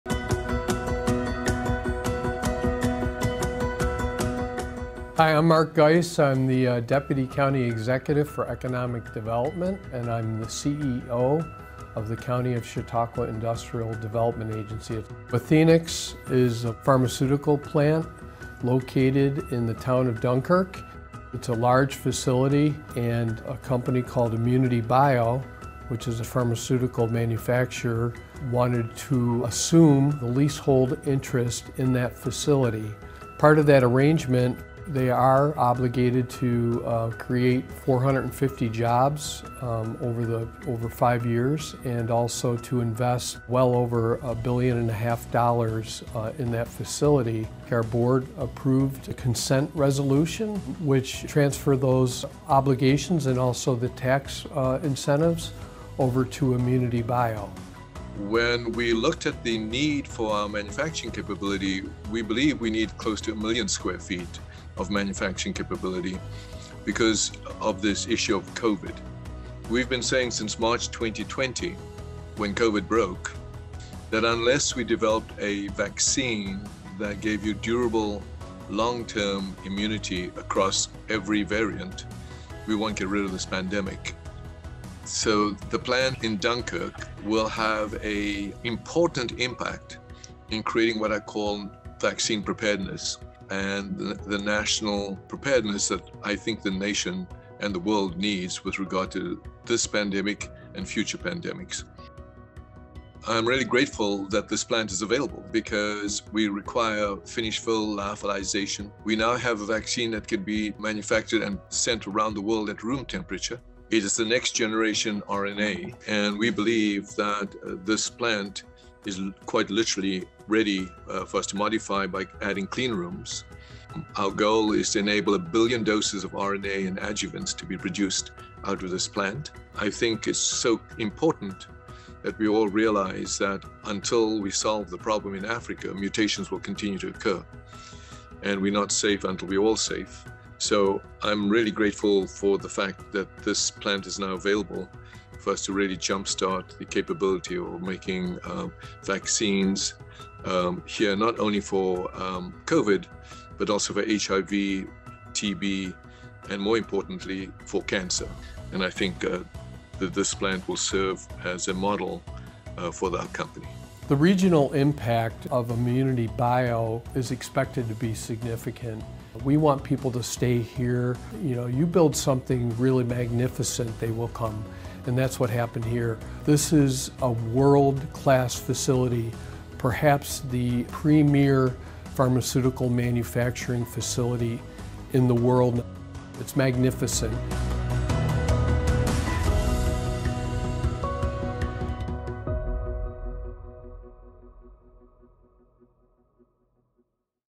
ImmunityBio NASDAQ: IBRX Assumes Leasehold Interest of ISO Class 5 Pharmaceutical Plant-Dunkirk, NY
Dr. Patrick Soon-Shiong, the Executive Chairman of ImmunityBio, a clinical-stage immunotherapy company (NASDAQ: $IBRX), met with the Board of Directors of the Chautauqua County Industrial Development Agency (CCIDA) on January 25, 2022. The pharmaceutical executive shared his vision for ImmunityBio and what role the ISO Class 5 pharmaceutical manufacturing space in Dunkirk, NY (Western New York, Chautauqua County) will have, creating 450 jobs within the next five years.

Athenex (NASDAQ: ATNX) had been the current leaseholder. “We’ve made the decision that you and your body have the immune system that we can activate,” he said. “We can change the paradigm of cancer, change the paradigm of infectious diseases and really use what’s called fusion proteins, cell therapy and vaccines to treat both cancer and COVID, HIV and TB.”

The 409,000 square feet manufacturing space will provide ImmunityBio with a state-of-the-art biotech production facility that will substantially expand and diversify ImmunityBio’s existing manufacturing capacity in the U.S., South Africa, and Botswana through its strategic collaborators.
“Our goal is to enable a billion doses of RNA and adjuvants to be produced out of this plant,” he said. Soon-Shiong said they have recently purchased a manufacturing plant in South Africa, where they will make the same product there for that continent, in the fight against COVID-19. “Until we solve the problem in Africa, mutations will continue to occur,” he said. They plan to be up and running very soon. “Quite literally, we needed this yesterday, so we are moving very fast, as fast as we can,” he said. They already have 100 people who were employed by Athenex and those individuals will continue. He said once they bring in the necessary equipment and information from their plants in Los Angeles and Colorado, they plan to “go on a hiring spree.”

He was invited to speak prior to the Board voting to pass a resolution to transfer the financial benefits and obligations from Athenex to ImmunityBio.

On February 15, 2022, Athenex announced the close of the sale of its leasehold interest in the manufacturing facility to ImmunityBio.